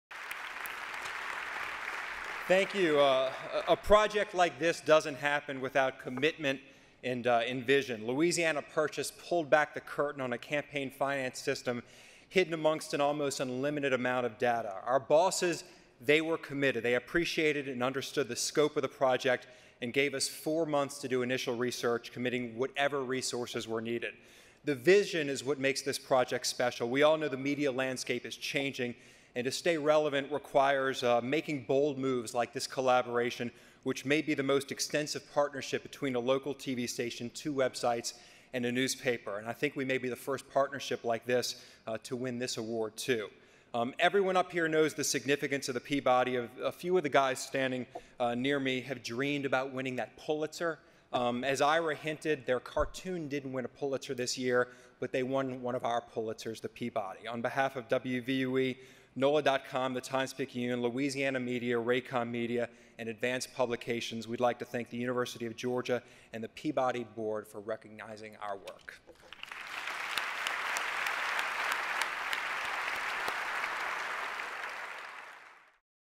Lee Zurik - Louisiana Purchased - 2013 Peabody Award Acceptance Speech
Winner 2013 | WVUE-TV & NOLA.com/The Times-Picayune

In their exhaustive investigation of campaign finance in the Pelican state, WVUE-TV and NOLA.com revealed illegal activities, dubious practices and feeble ethics enforcement. The New Orleans TV station and the website, an arm of the Times-Picayune newspaper, devoted thousands of employee hours to sifting through almost a million campaign documents to figure out who gave how much to whom and what they got in return. One revelation was that nearly a third of the $209 million pumped into Louisiana campaigns between 2009 and 2012 came from less than one percent of the donors. What made Louisiana Purchased stand out even more, however, is the verve with which it was reported. The graphics, starting with the series’ logo, a bar-coded Louisiana map, are imaginative, even amusing, and on target. So is the writing. “If money drives politics,” WVUE reporter Lee Zurik says early on, “we’re about to show you who’s riding shotgun.” Other news organizations would do well to emulate the project’s methods –- not just its commitment but its attitude. For its diligent, unusually accessible exposé of a state’s labyrinthine campaign-finance system, Louisiana Purchased receives a Peabody Award.